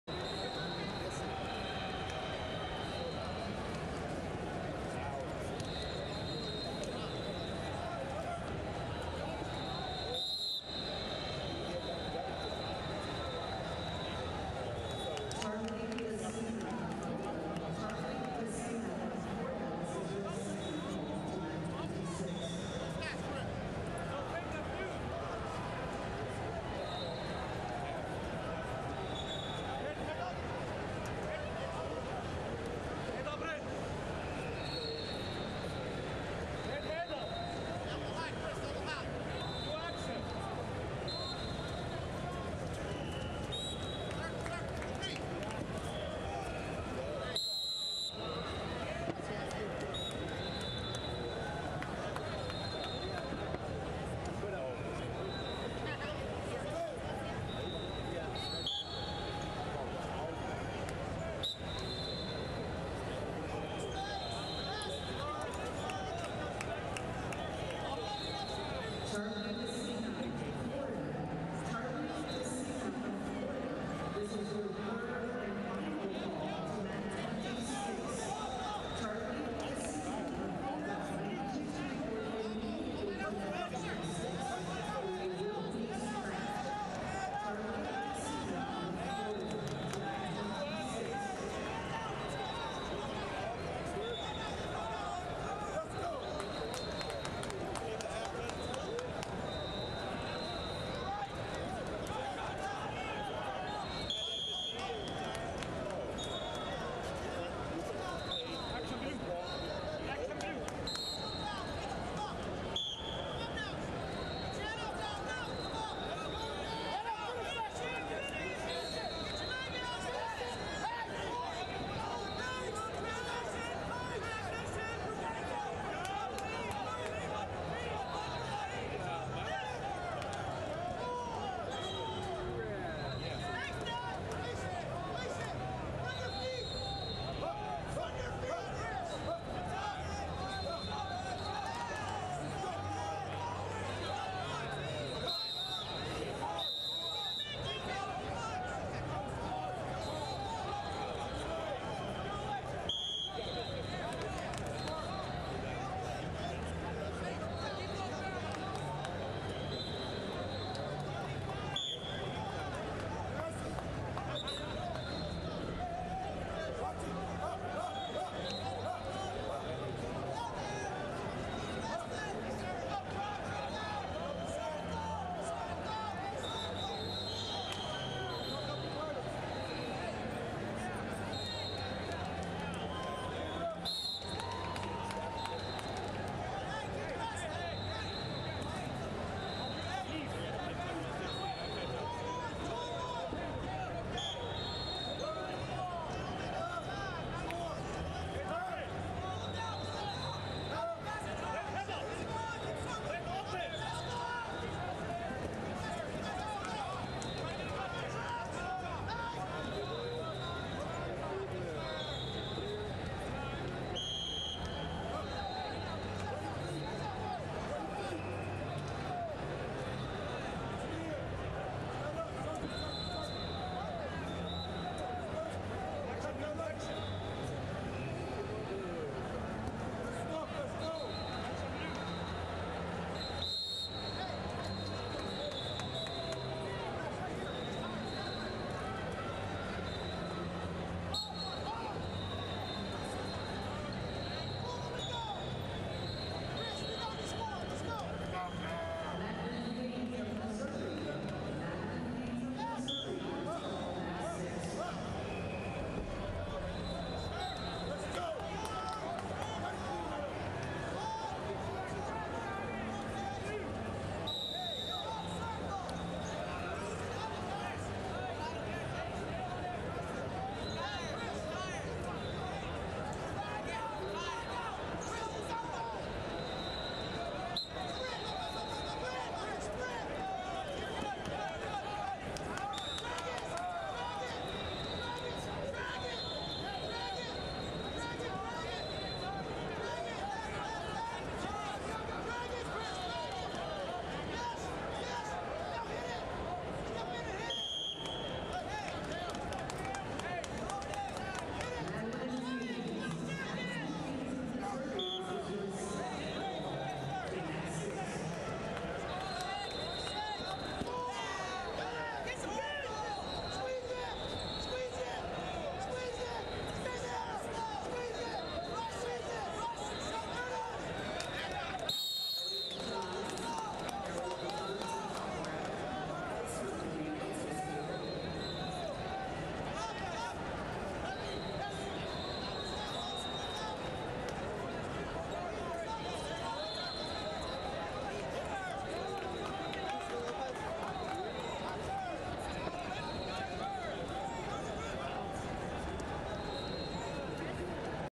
113 Lbs Round Of 64 - Christopher Lopez, Arizona Vs Easton Mull, Pennsylvania 939c
2022 USMC/USAW Junior And 16U National Championships - 113 Lbs Round Of 64 - Christopher Lopez, Arizona Vs Easton Mull, Pennsylvania 939c